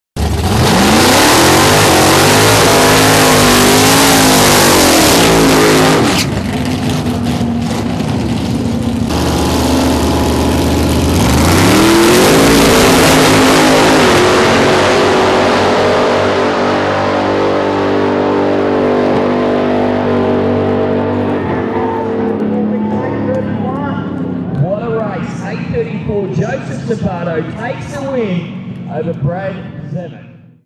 APSA Round 3 - Pro Street Blown final
With most of the big guns running Outlaw 10.5 or Outlaw Radial the Pro Street Blown final was a bit slower that usual. It was a close race though with the holeshot being the deciding factor.